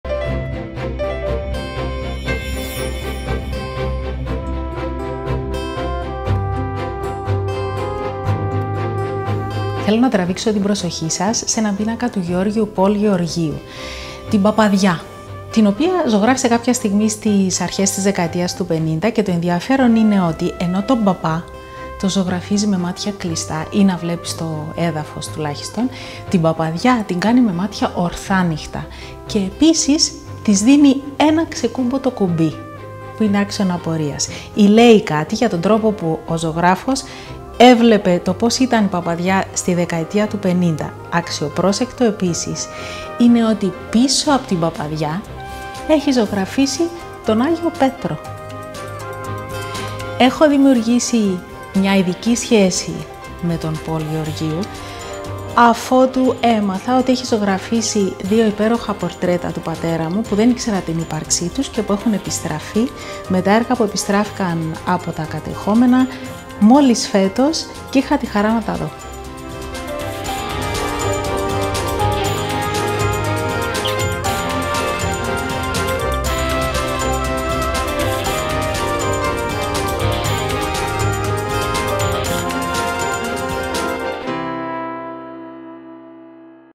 I love this painting / Marina Maleni
Rediscover the A. G. Leventis Gallery!

With the online action 'I love this painting!’ we invite visitors to share their favourite painting from our Collections with the public.